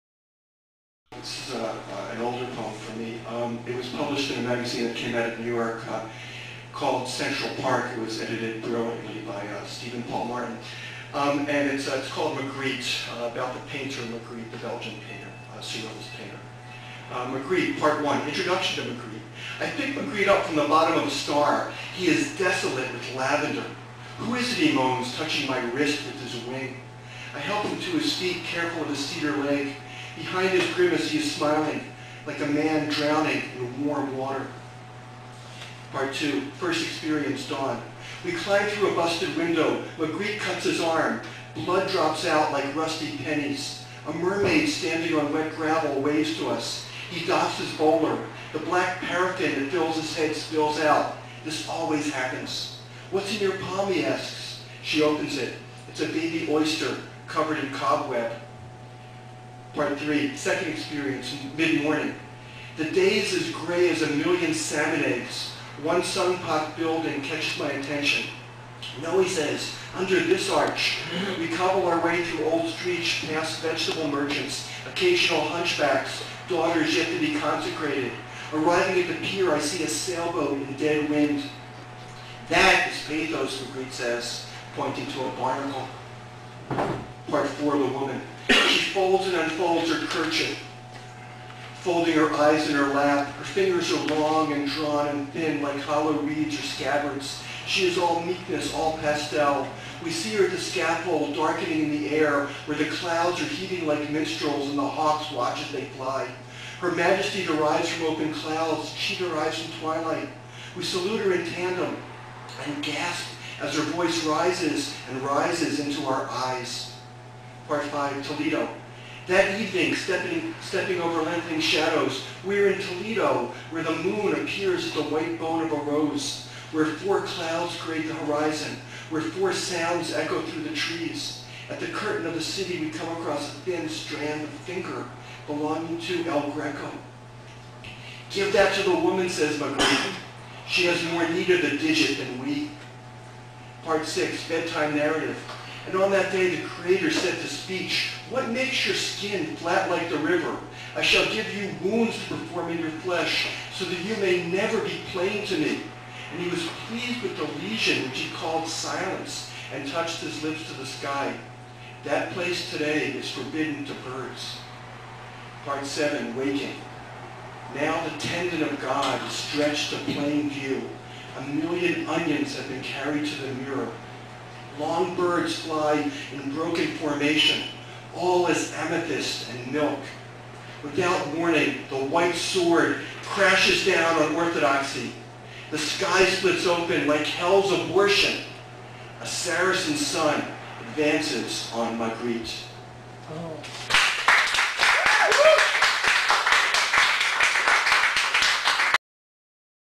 Bill Yarrow reading "Magritte"
Bill Yarrow reading "Magritte" at The Cafe on July 12, 2011.  "Magritte" was first published in Central Park: A Journal of the Arts and Social Theory. It  appears in Pointed Sentences (BlazeVOX, 2012).